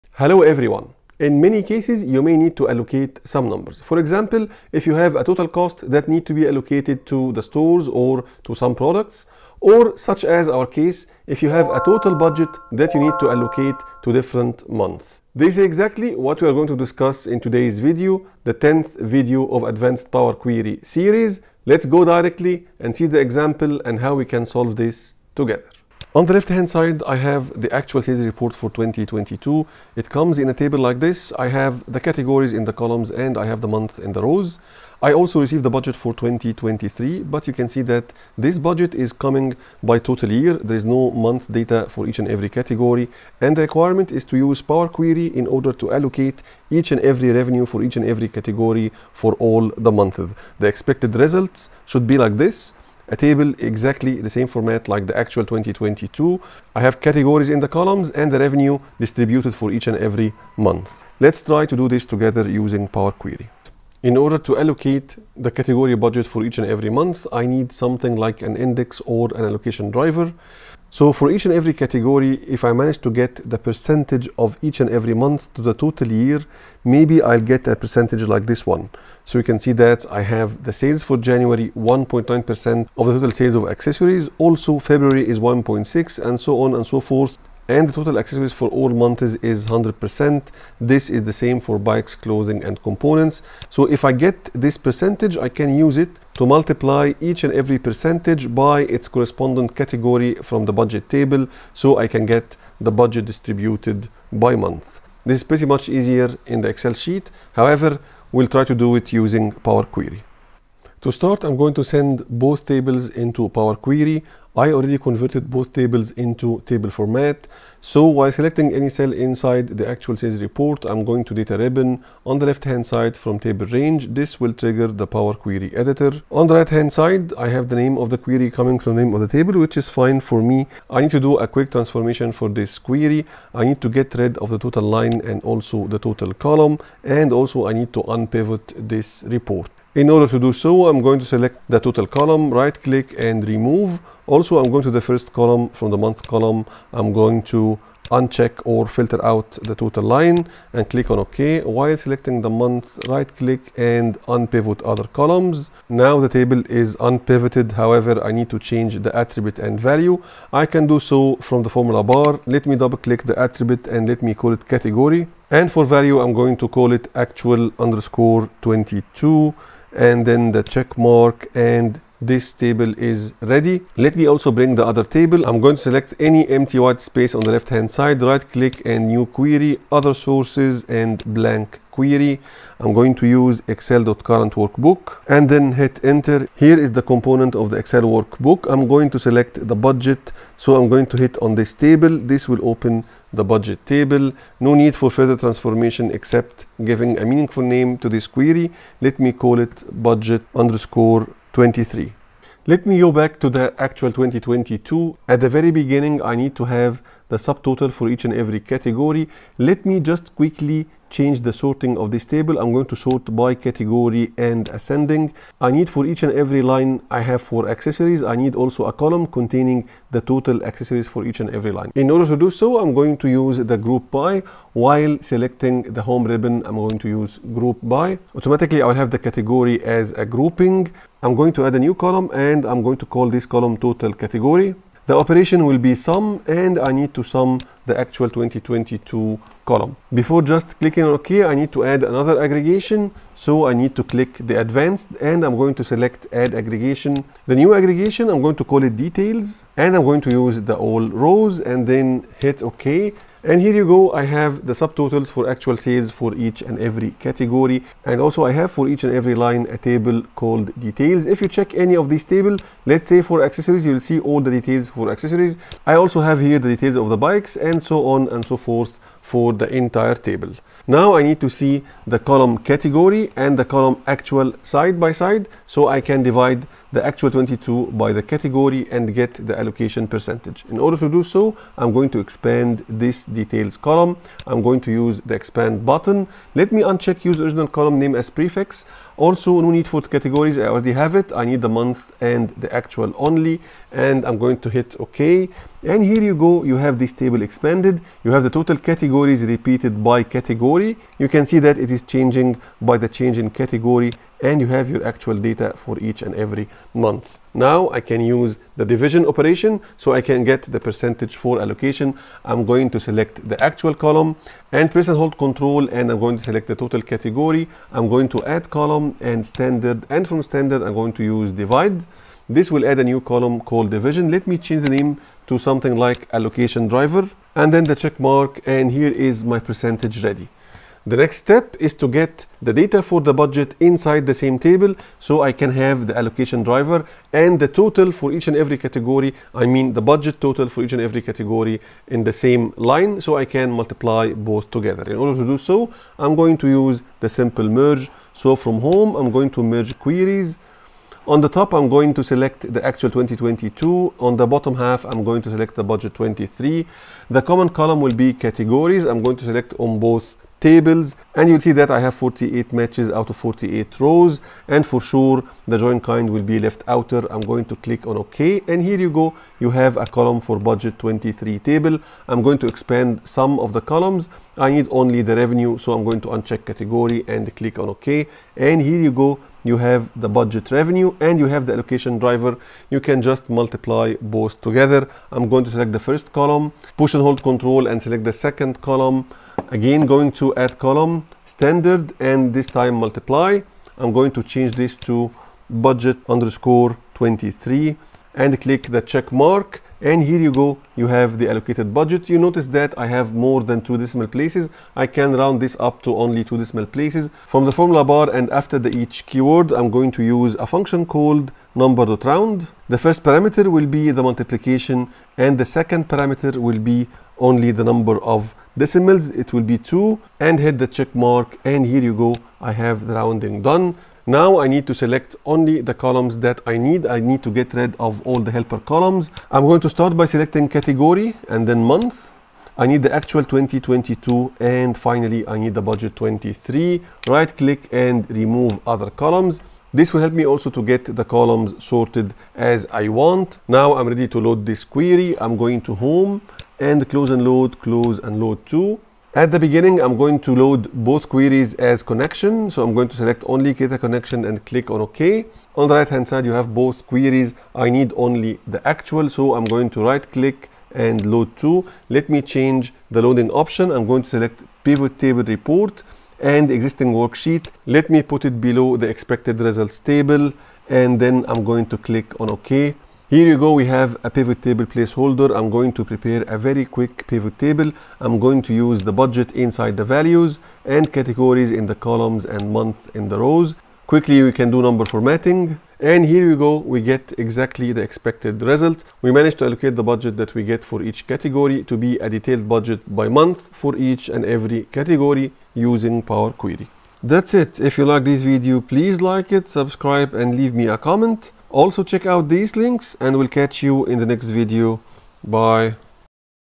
APQ10 | Budget Allocation using Power Query| Group By All Rows | Advanced Power Query
Chapters:
0:00 Intro
0:25 Budget Allocation example
1:03 Solution methodology
1:50 Send act_22 to Power Query
2:13 Remove unnecessary rows and columns and unpivot the table
2:53 Send bud_23 table to power query
3:22 Use group by (all rows) to calculate subtotals while keeping details
5:07 Calculate allocation %
5:30 Use merge queries to lookup budget numbers
6:18 Calculate allocated budget & remove unnecessary columns
7:23 Load result query to a pivot table